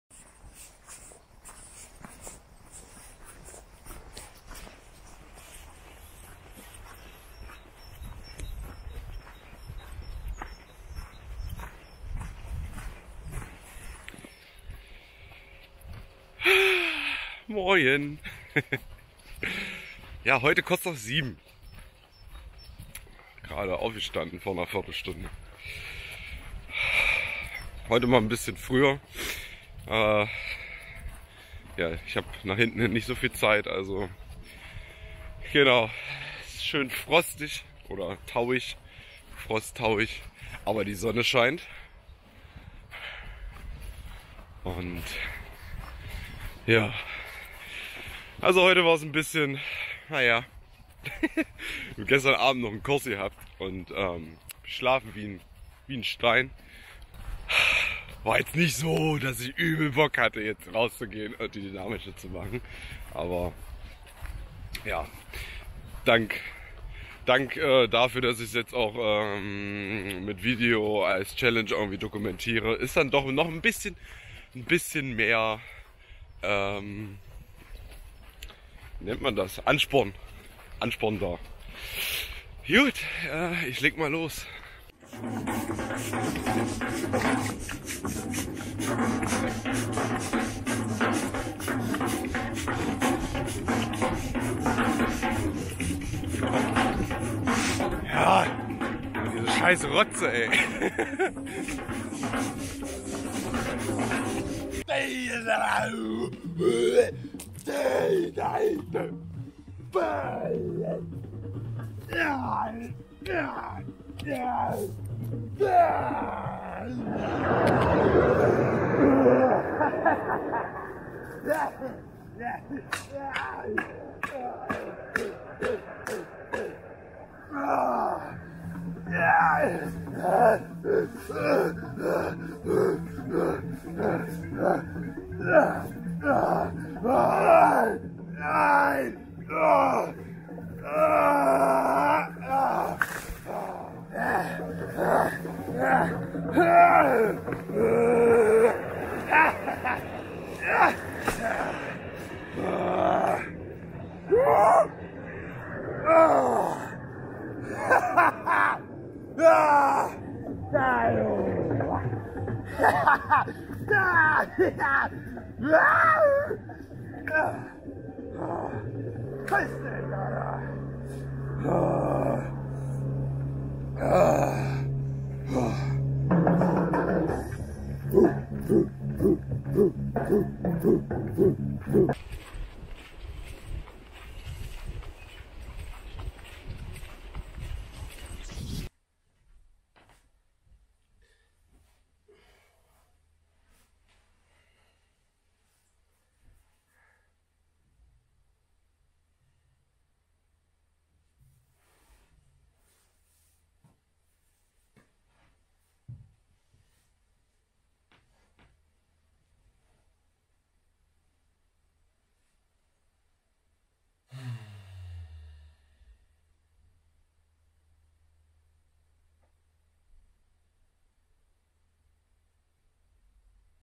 Tag 2 - OSHO Dynamische Meditation (21 Tage Challenge)
Die Dynamische Meditation begleitet mich schon einige Jahre. Sie kann seelisch/mentalen alten Rotz zu Tage bringen und dann transformieren/auflösen. Man fühlt sich lebendiger und entspannter, da alles mal Raum haben darf, was wir uns sonst verbieten. Allerdings habe ich es nie geschafft die empfohlene Kur von 21 Tagen durchzuziehen. Das ändert sich ab Heute! Aufgrund einer Aufstellungarbeit bekam ich die Hausaufgabe diese 21 Tage zu absolvieren und nehme euch auf diese Reise mit.

Der zweite Tag!

Wenn du das liest, fühlt dich umarmt und geliebt. Danke dass es dich gibt!
Liebe Grüße
Paul